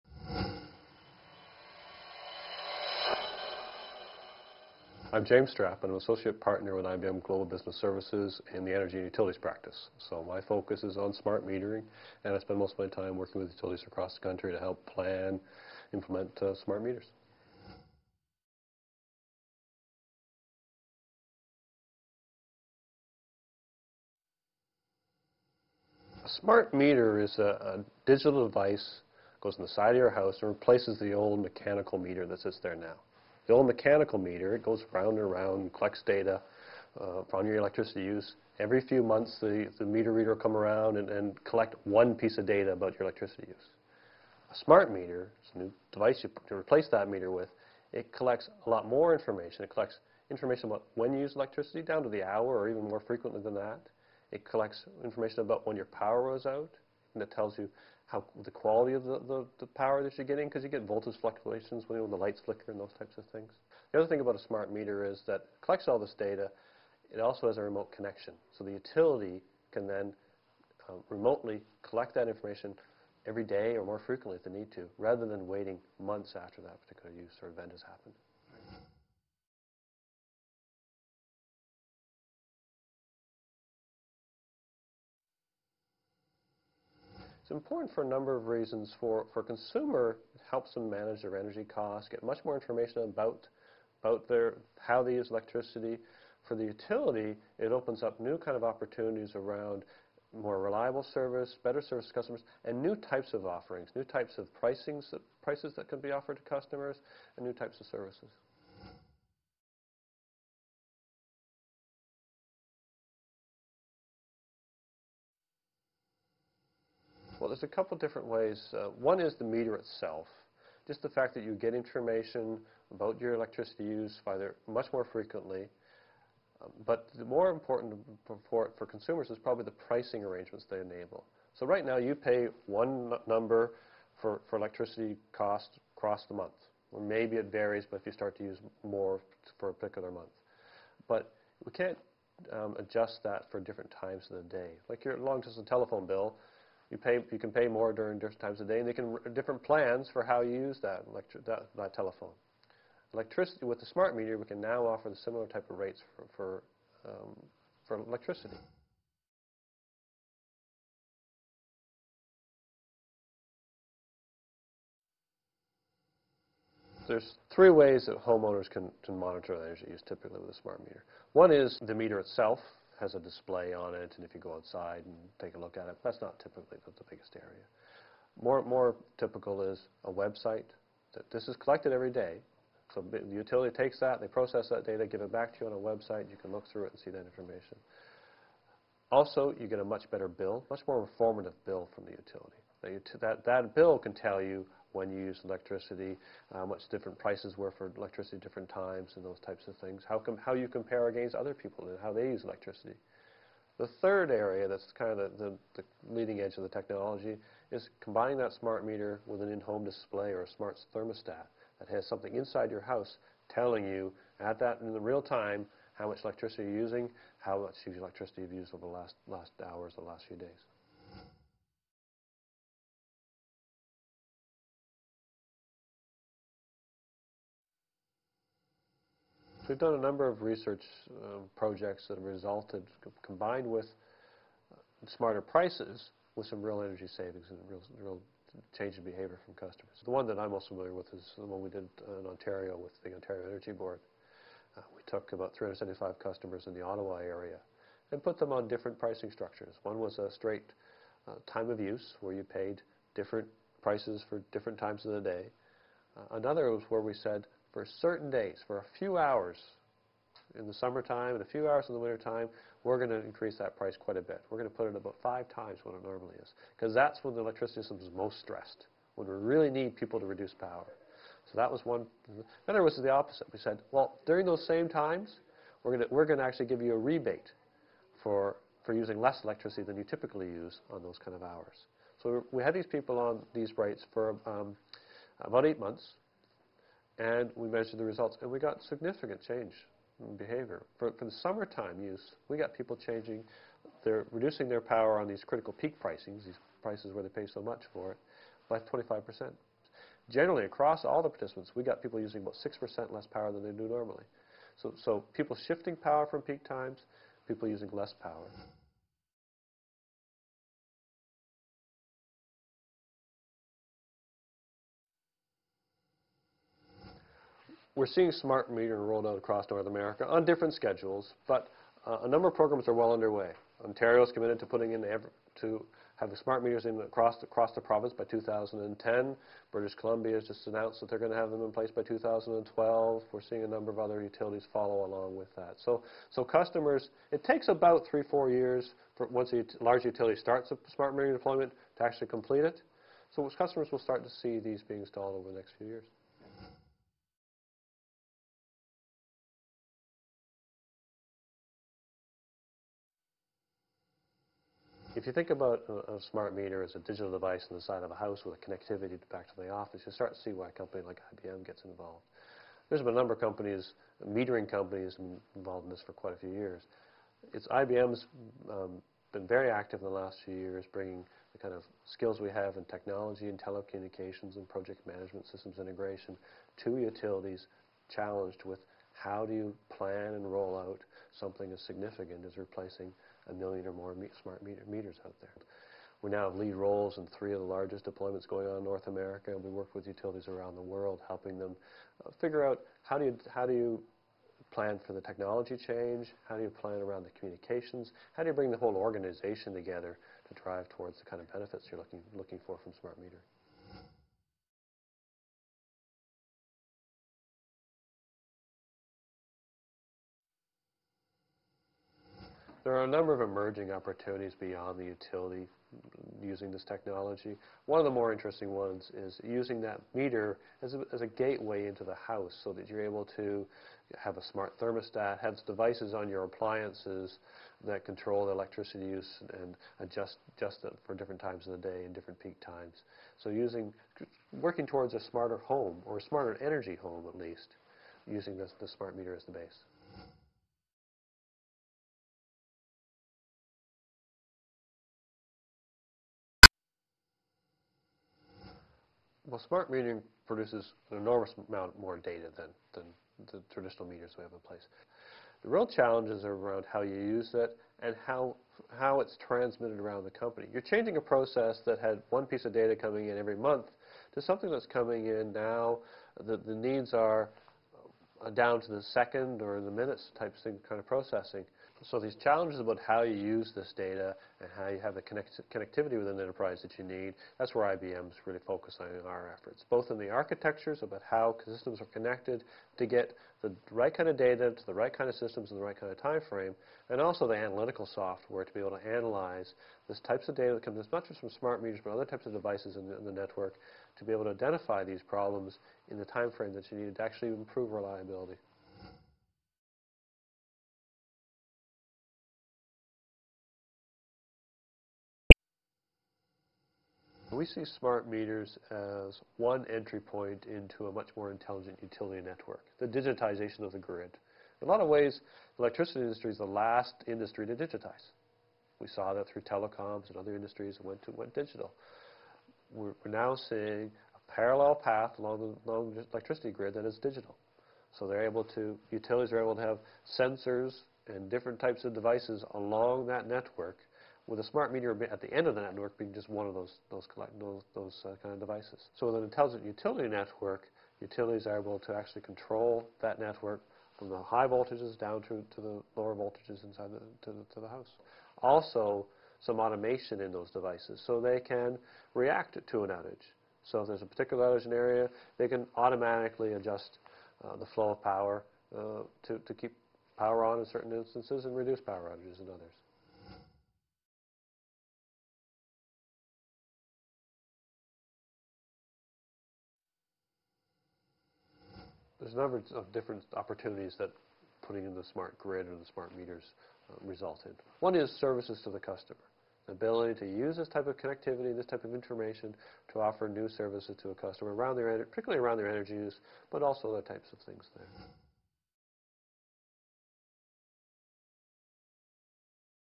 Innovation in Action: Power to the People
Smart metering will give electrical utilities more control over their transmission networks -- and will give you more information on how you use electricity. How will this affect consumers, businesses and the environment? And how can you benefit? In an interview for Maclean's readers, James Strapp, Associate Partner with IBM Global Business Services Energy and Utilities Practice, explores the new frontier of smart metering.